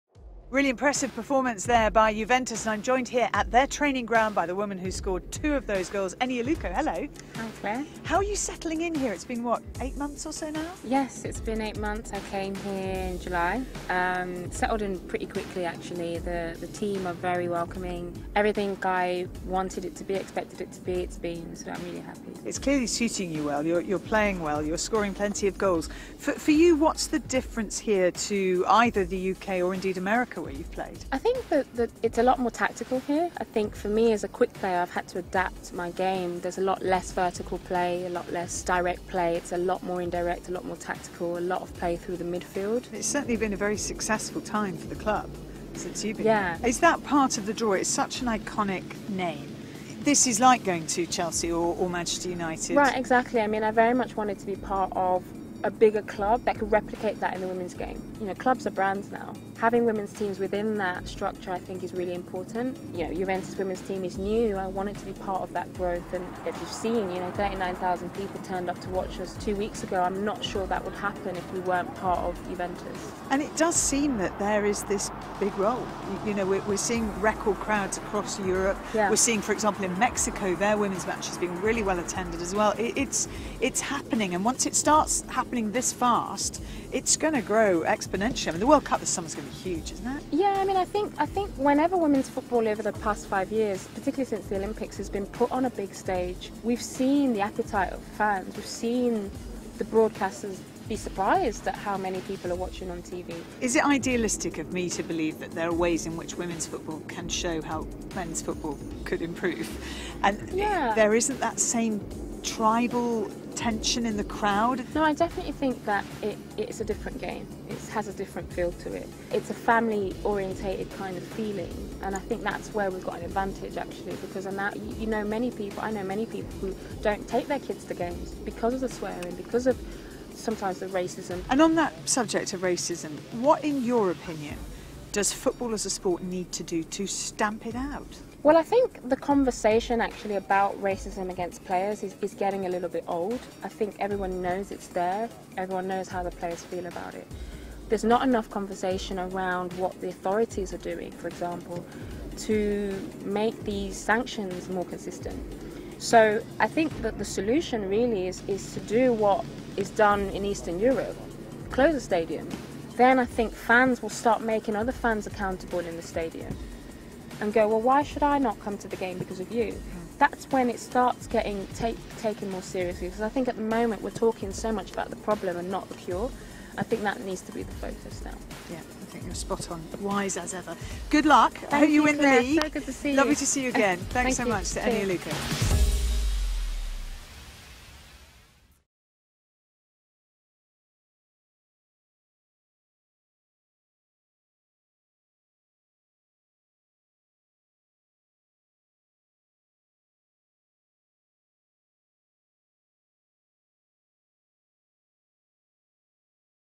Eniola Aluko on New Team & Racism in Football | Women's Football World with Coca-Cola
Clare Balding is joined by Juventus forward Eniola Aluka for a conversation on her new team, adapting, racism in football, and more.